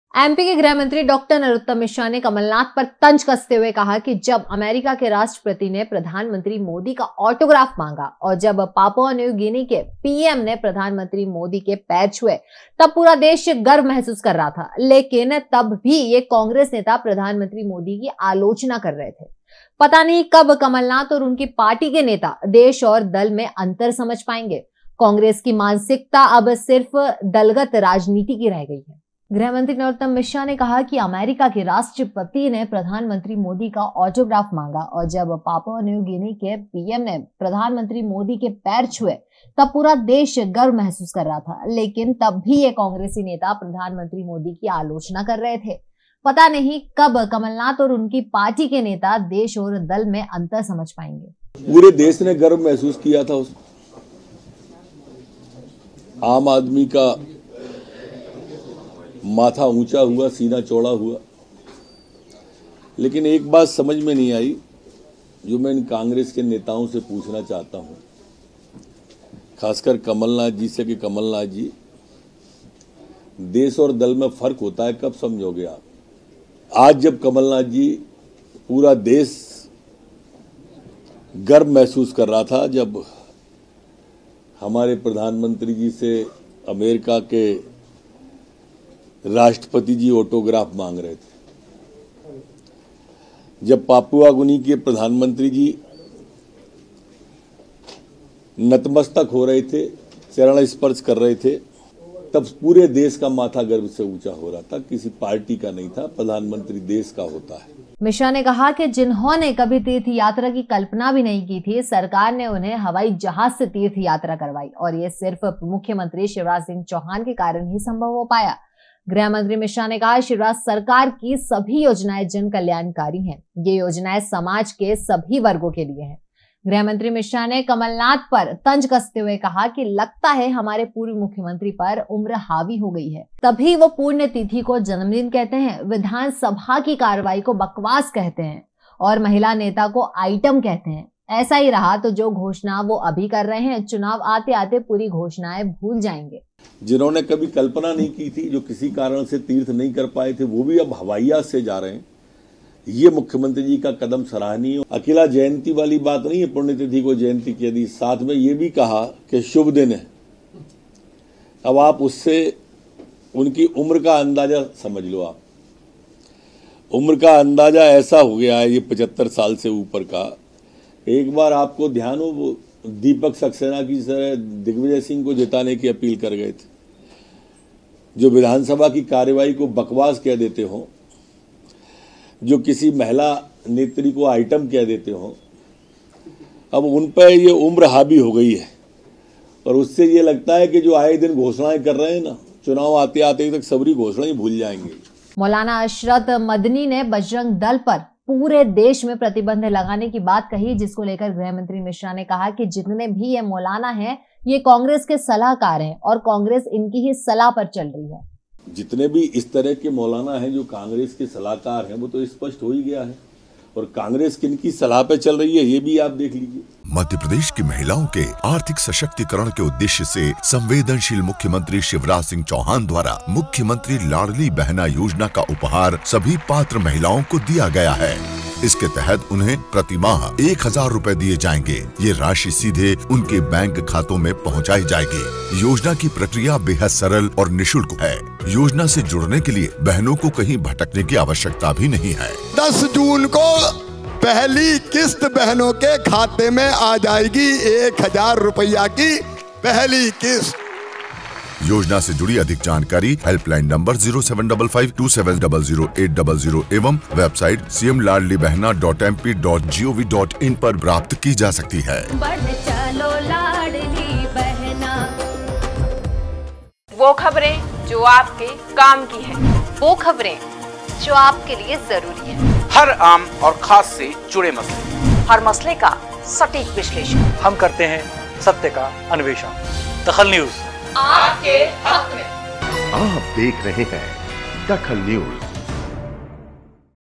DAKHAL NEWS गृहमंत्री ने नरोत्तम मिश्रा ने साधा कमलनाथ पर निशाना
गृहमंत्री ने नरोत्तम मिश्रा ने साधा कमलनाथ पर निशाना
मिश्रा ने कहा देश और दल में कब अंतर समझेंगे नाथ